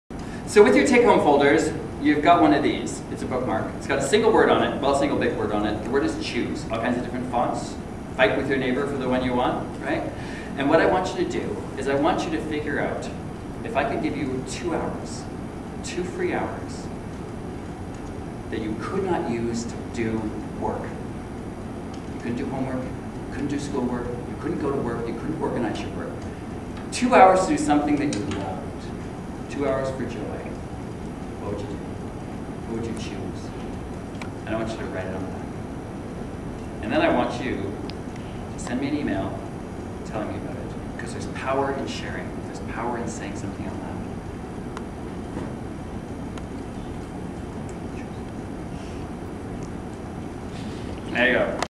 Robert Corbett Motivational Speaker and Humourist TICK TALK Choose
Buried under your to do list? It's time to fix the time crunch with Rob's TICK TALK!

Rob teaches acting at four colleges, is studying full time toward his MFA in Creative Writing, freelances, has two young children and a partner who works nights and weekends.

He still makes time to connect with friends, read, stay fit, write, amass
99 496 points (and counting) on Farmville, and floss.

He isn't organized by nature, does not multitask well and isn't one of those people who can thrive on four hours of sleep a night.

He's just a regular guy who's figured out how to make time for the life he wants and is passionate about helping you do the same.